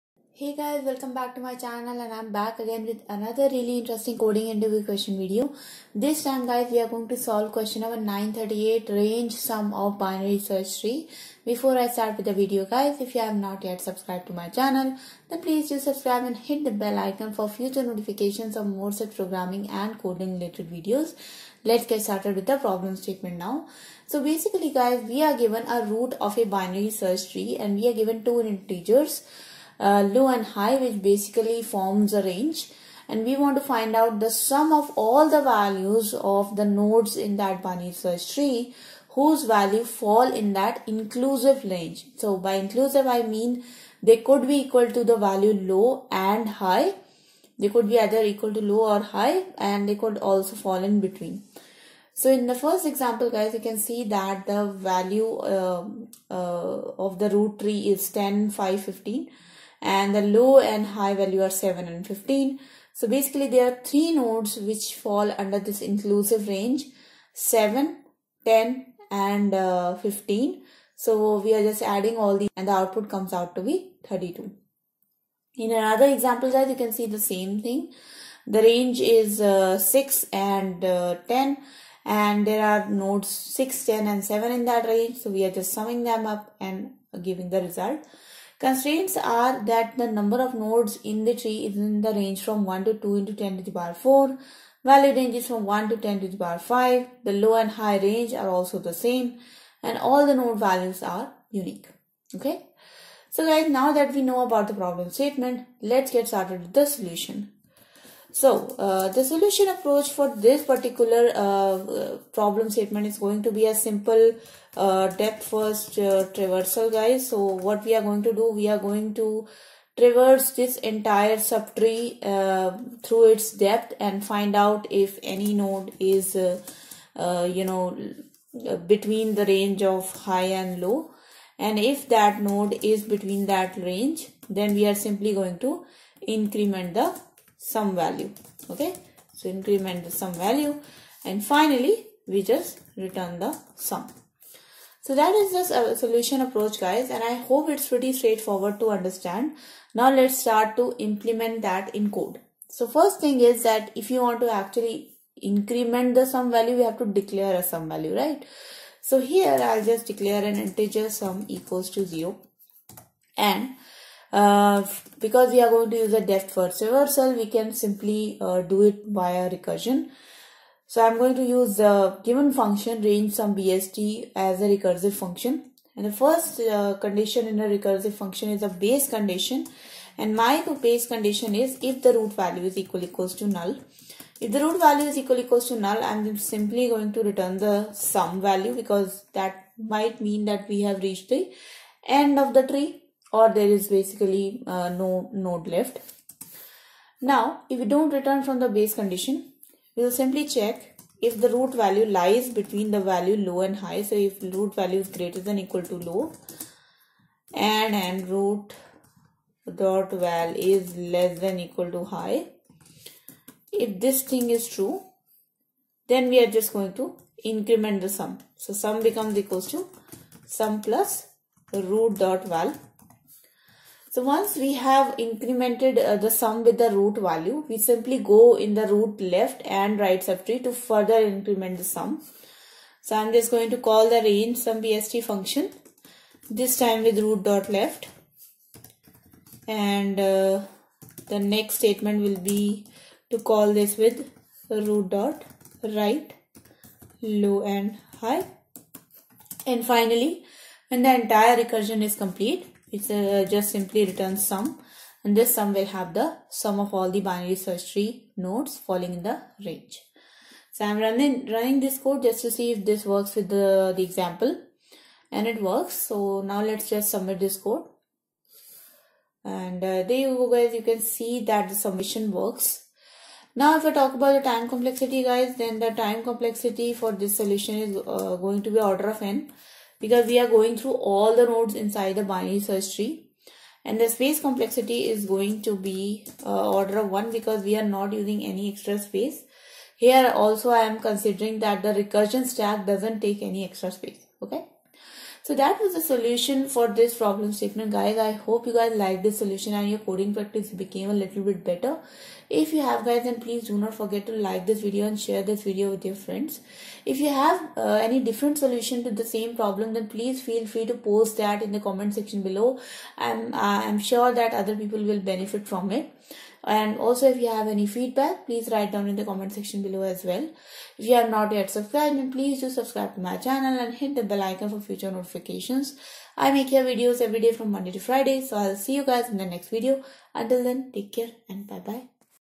Range Sum of BST | Leetcode 605 | Amazon Google Facebook interview question
Range Sum of BST - a top coding interview question asked in Amazon, google, Facebook and also from Leetcode easy list. Please provide feedback in comments below and let me know if I should any other topics as well

0:00 Introduction
0:23 Problem statement
1:44 Approach
2:32 Code
4:33 Complexity
5:03 Conclusion